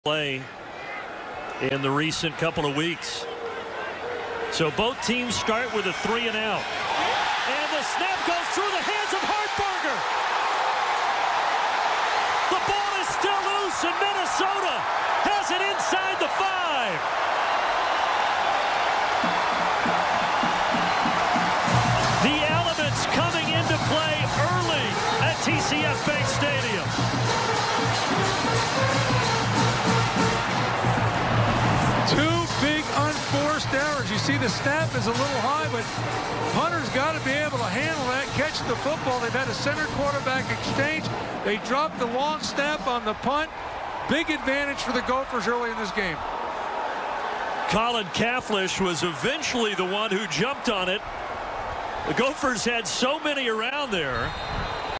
Gophers Recover Mishandled Michigan State Punt
Minnesota ends up with great field position after recovering the mishandled punt snap by Michigan State.